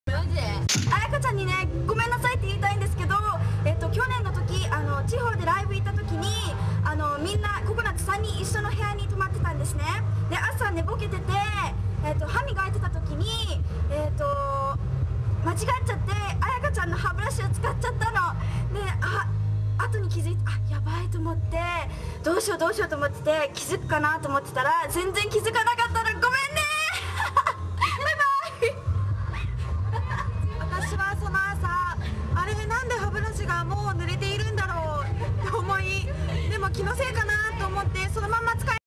[Hello! Project] Coconuts Musume Confessions
Mika and Ayaka confess some things they done in the past :3